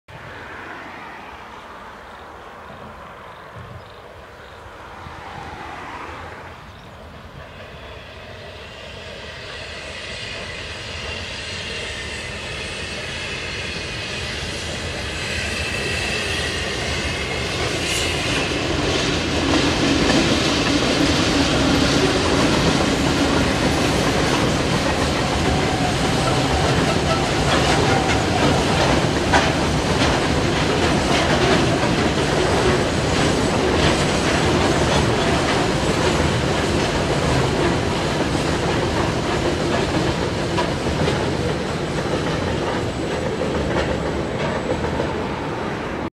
TasRail TR07 TR02 #46 Coal Train Longford Viaduct
09/10/2014 TasRail's coal train heading over the Longford Viaduct on it's way to Railton. Filmed at 1:36/13:36